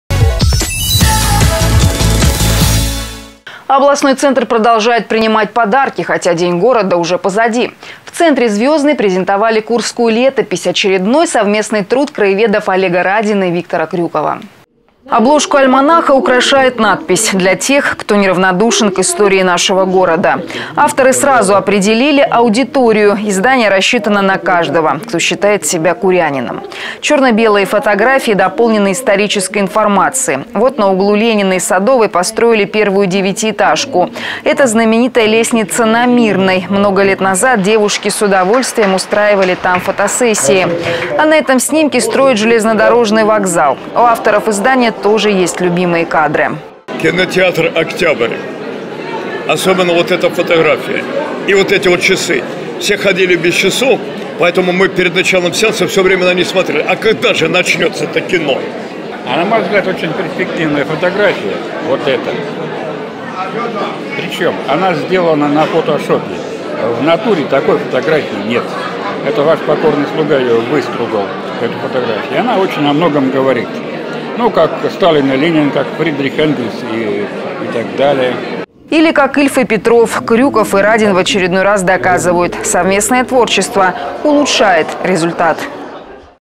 День города позади, но Курск продолжает принимать подарки
В центре «Звездный» презентовали «Курскую летопись» — очередной совместный труд краеведов Олега Радина и Виктора Крюкова.